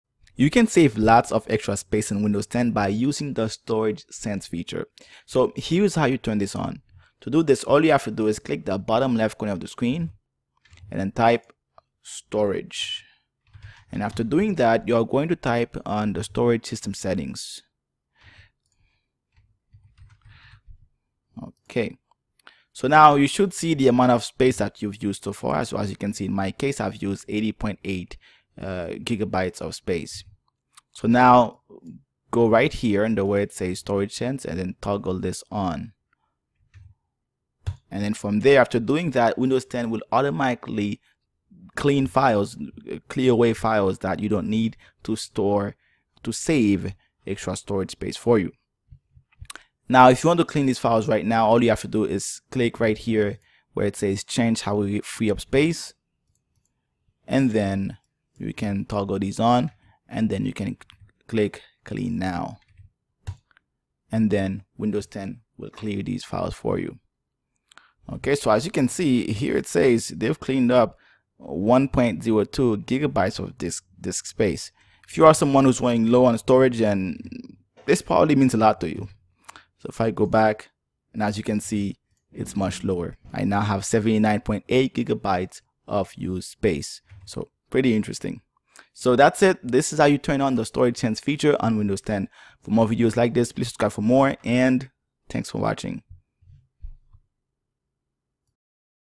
How to Use Storage Sense to Free Up Space on Windows 10
This video shows how you can use the storage sense feature on windows 10 to release or free up storage space from your hard drive or solid state drive (SSD).